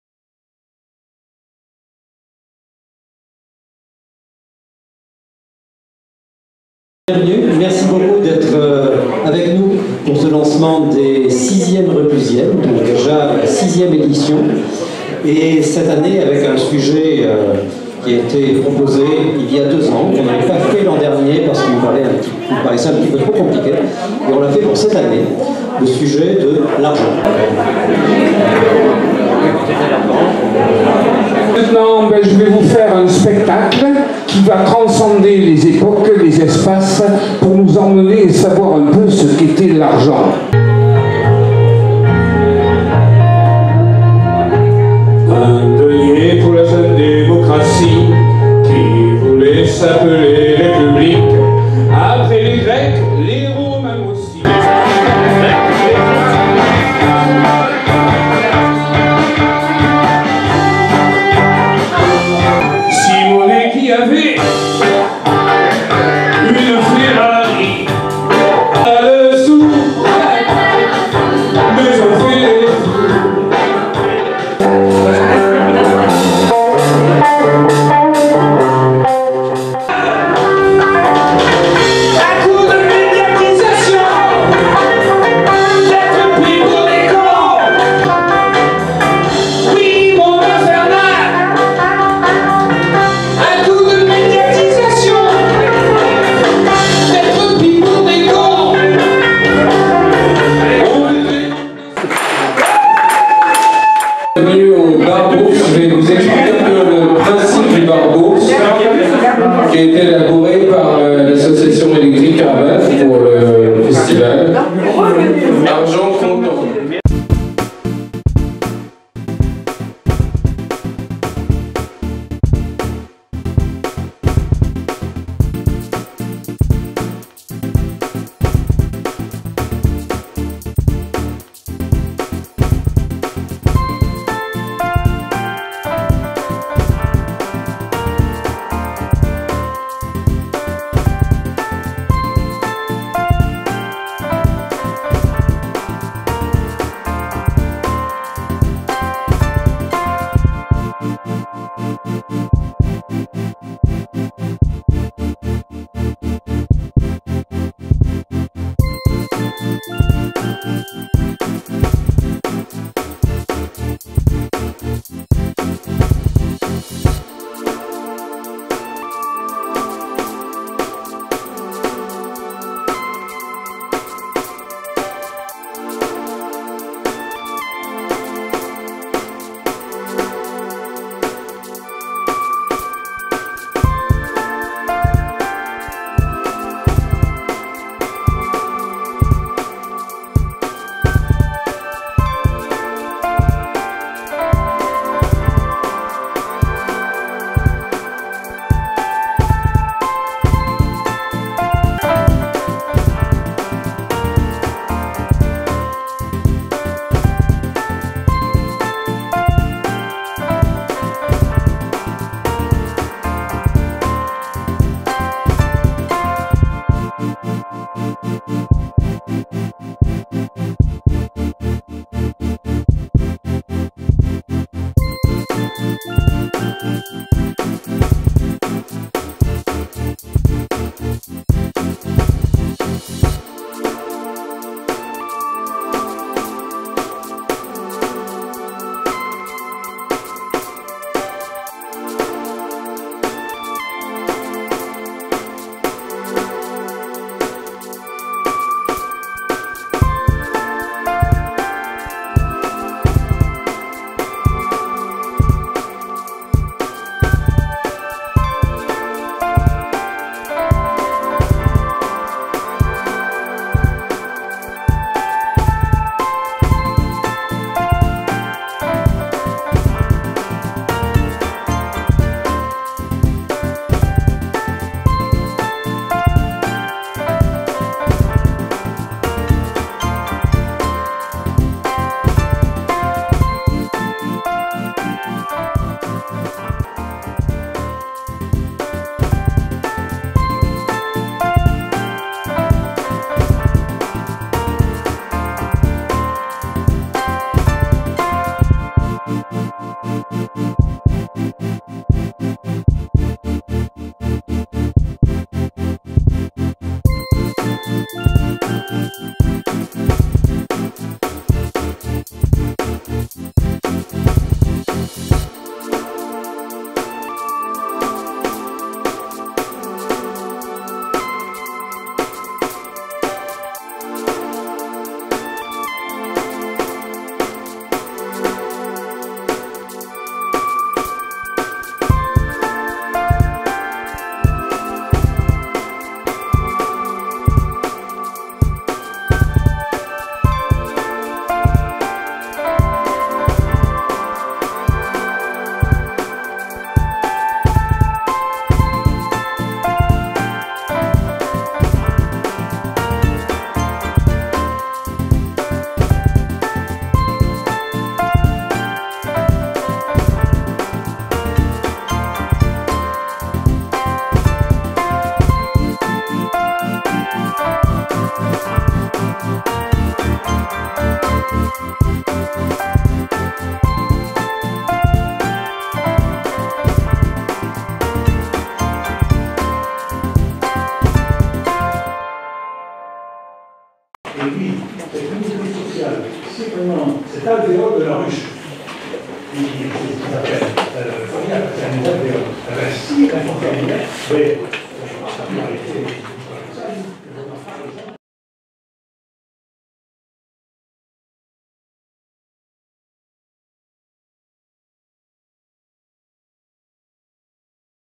FESTIVAL DES RECLUSIENNES 2018
Du 4 au 8 juillet 2018, a eu lieu le festival de la pensée "Les Reclusiennes".
"Argent Content" était le thème de cette 6ème édition.
Ateliers, débats, spectacles et concerts, ont rythmé cette semaine de rencontre entre chercheurs et population.